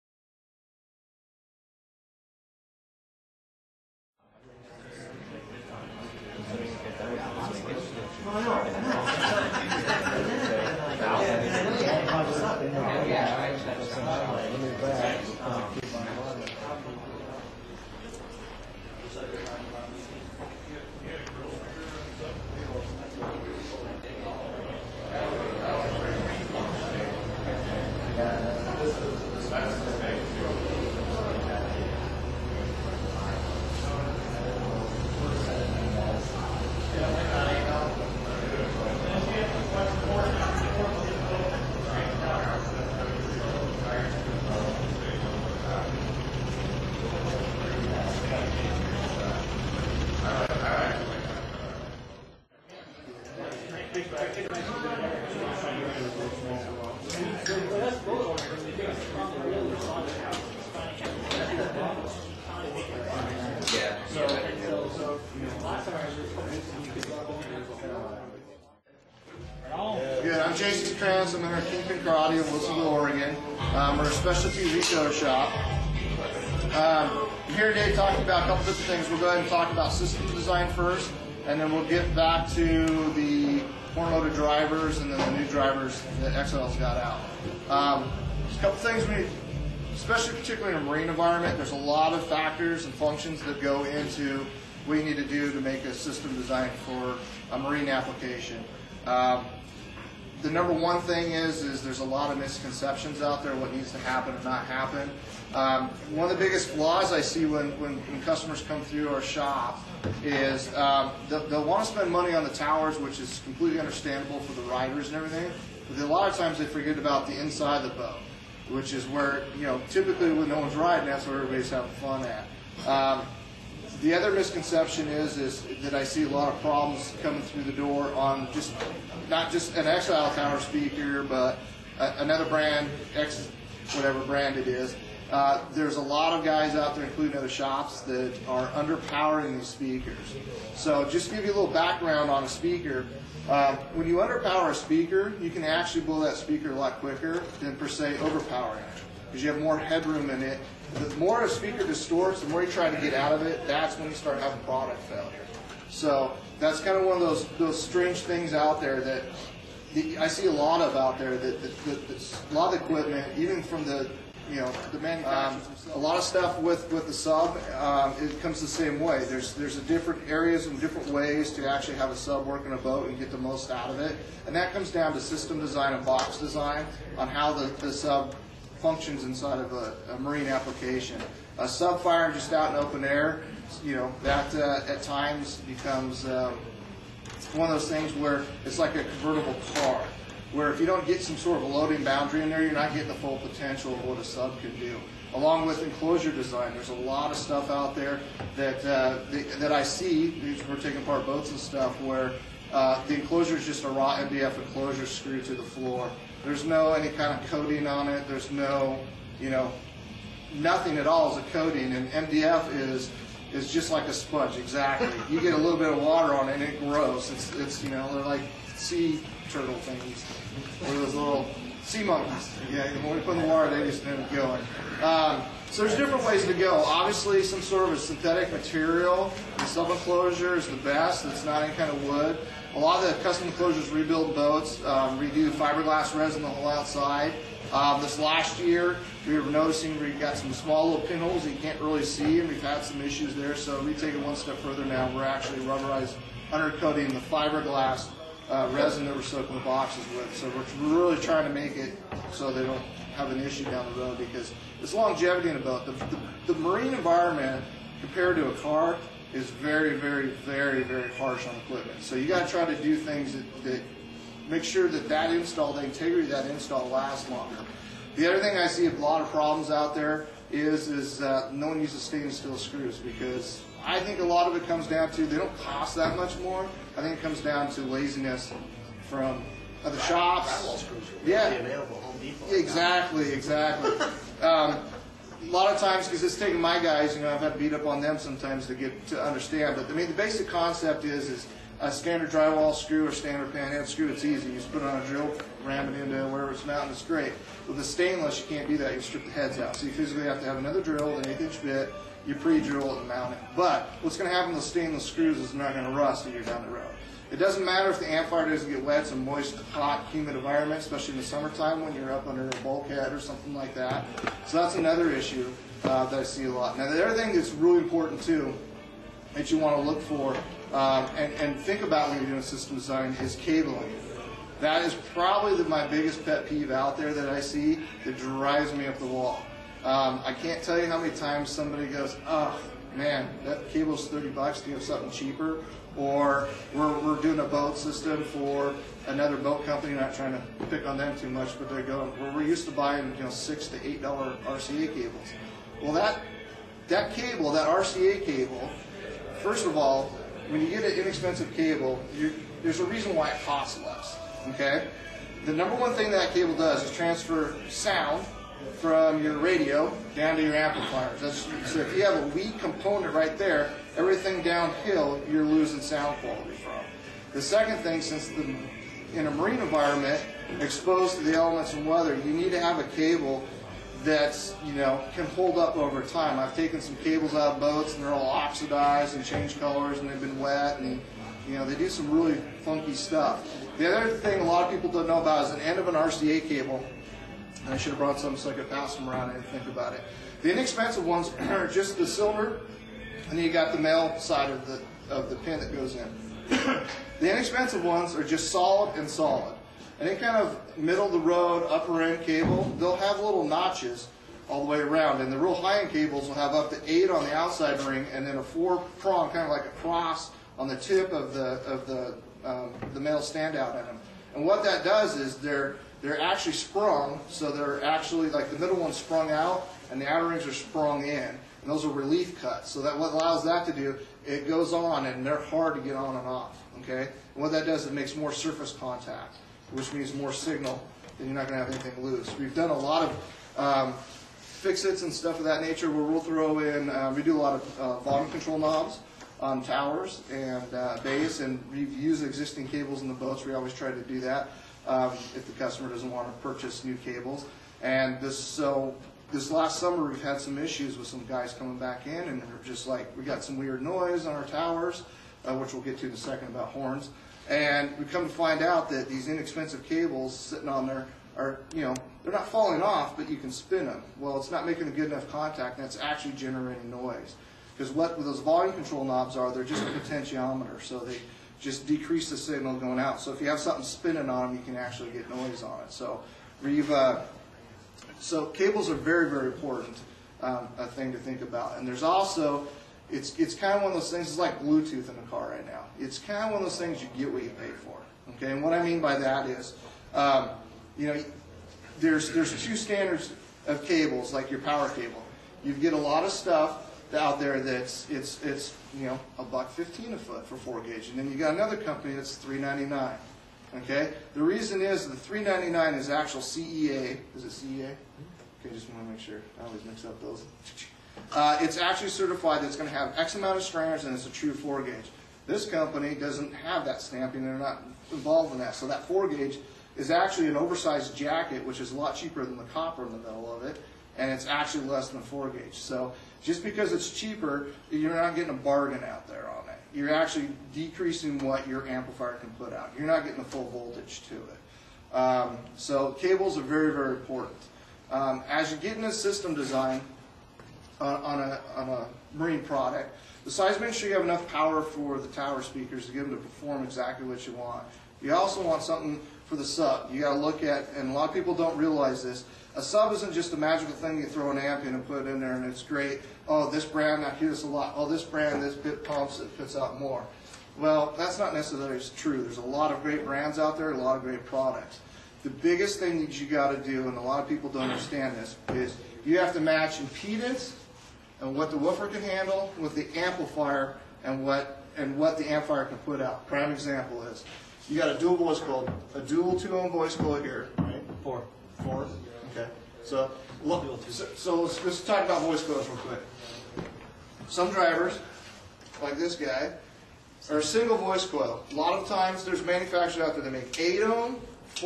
Exile TechTune NW Part 1.wmv
Had a good time with boaters from the NW. This is a run down on system design and audio building in the marine environment. Good tech info for guys that want to learn a few things.